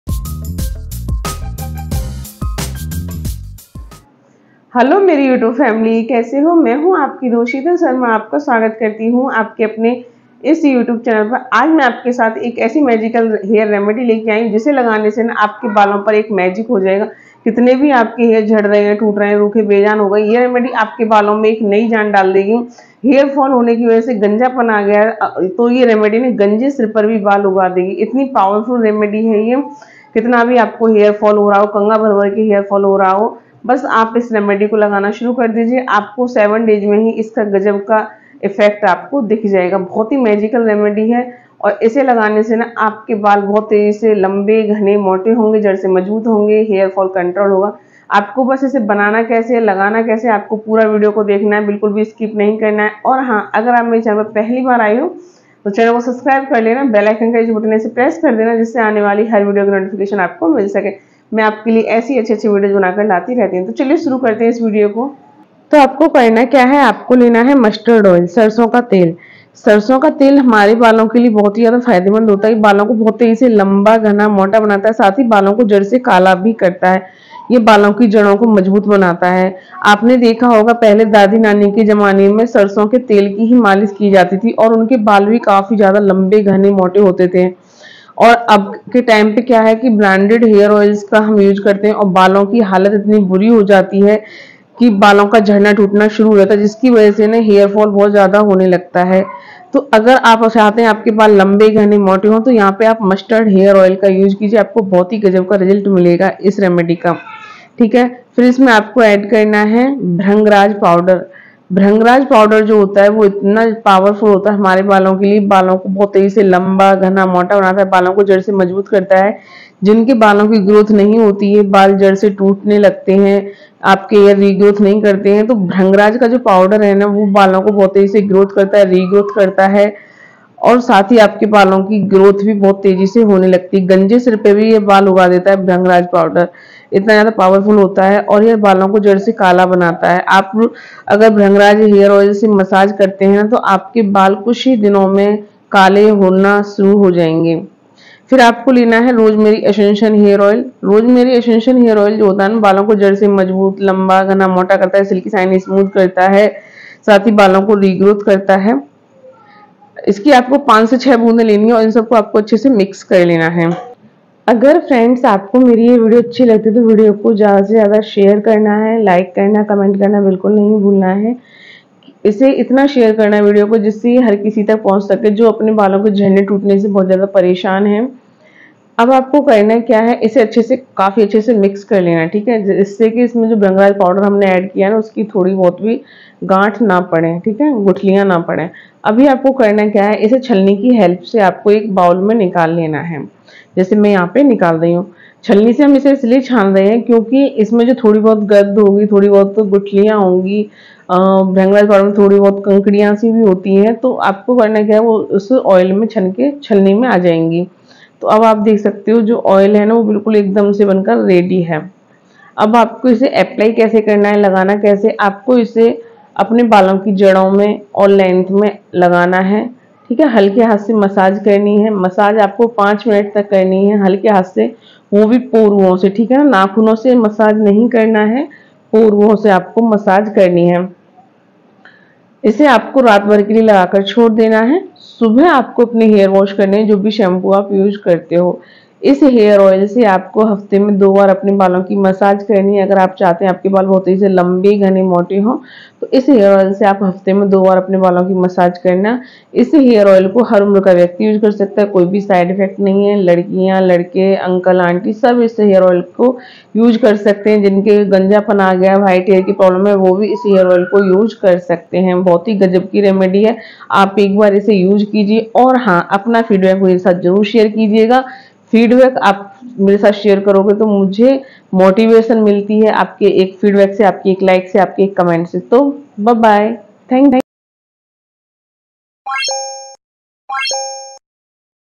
कसम से एक बार लगाकर देखो बालों की लंबाई इतनी बढ़ेगी कि कटवाते रह जाओगे / Super Fast Long Hair
कसम से एक बार लगाकर देखो बालों की लंबाई इतनी बढ़ेगी कि कटवाते रह जाओगे / Super Fast Long हेयर

Welcome Sheetal Hacks & Tips .In today's video, I am sharing a magical hair oil which will increase the length of hair 2 times faster.  Just mix this medicine in mustard oil and apply it and your hair will become long, thick and strong.  This oil will make hair grow even on bald head, it will stop hair fall and breakage, it will give new life to even the most dry and lifeless hair.  So watch the video completely and do not skip it.  Thank you❤️

Please keep your love with me and support me.❤️🥰